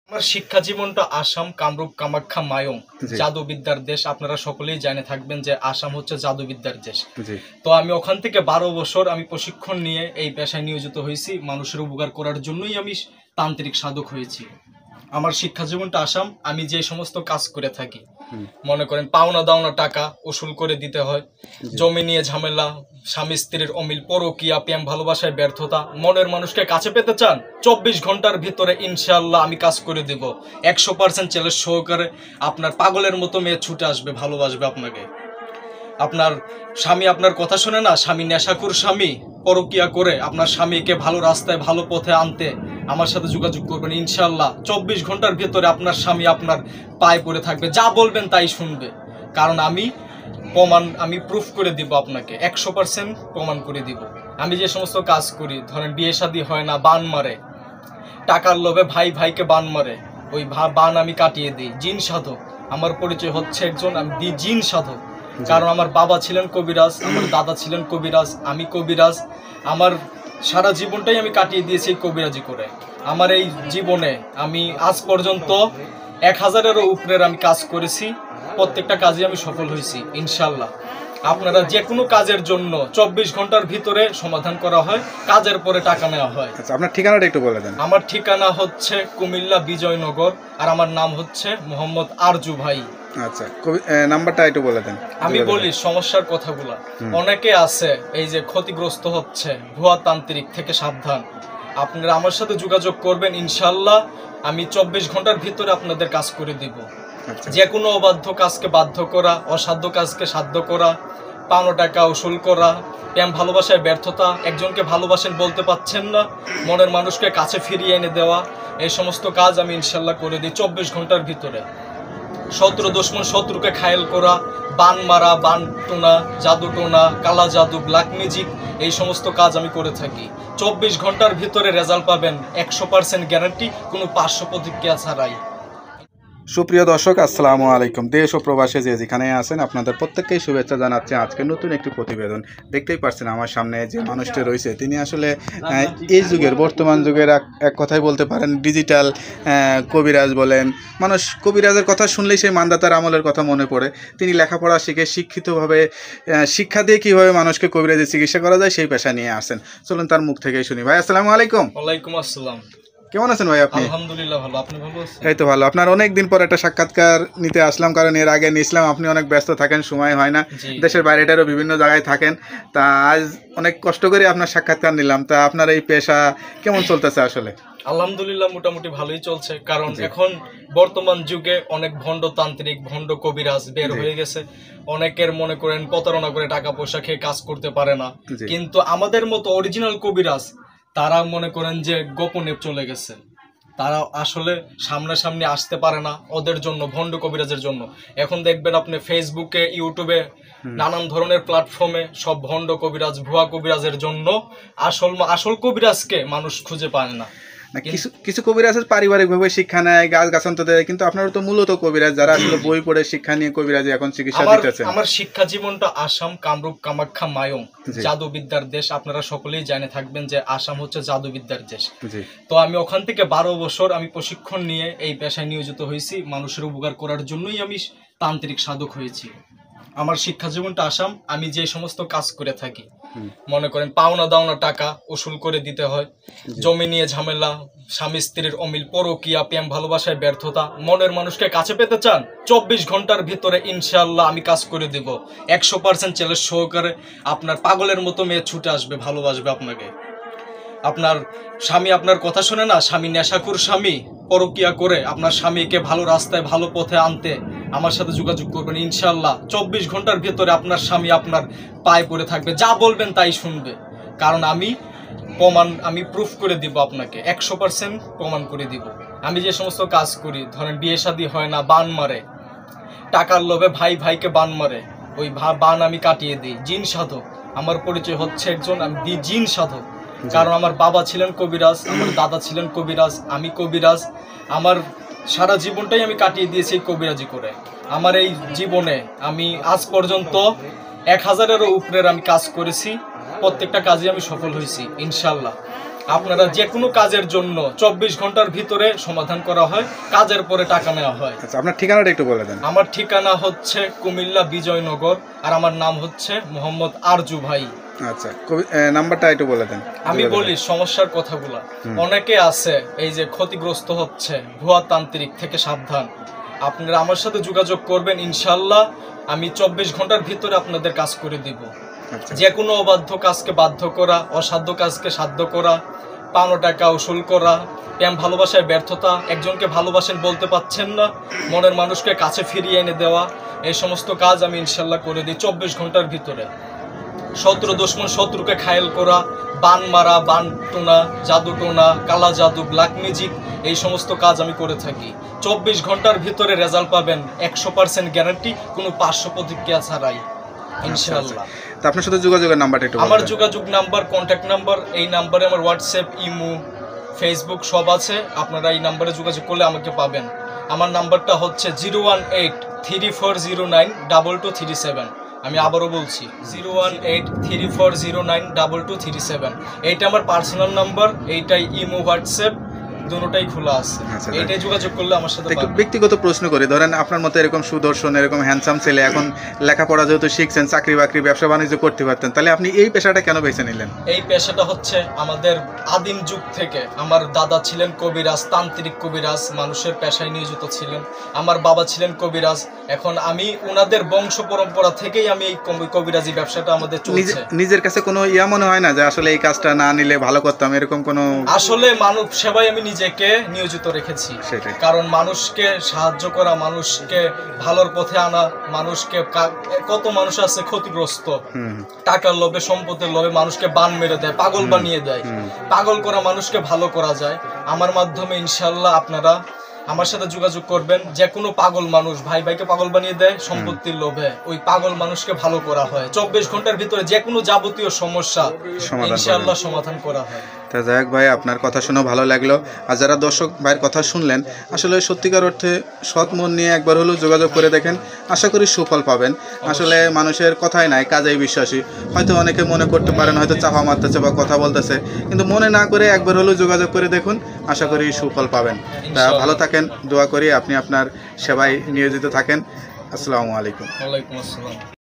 বাংলাদেশে এই প্রথম অরিজিনাল জিন সাধক তান্ত্রিকের দেখা যিনি ২৪ ঘণ্টার মধ্যে যে কোন কাজ করতে পারেন।
বাংলাদেশে এই প্রথম অরিজিনাল জিন সাধক  তান্ত্রিকের দেখা পেলাম যিনি ২৪ ঘণ্টার মধ্যে যে কোন কঠিন  কাজ করে দিতে পারেন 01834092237,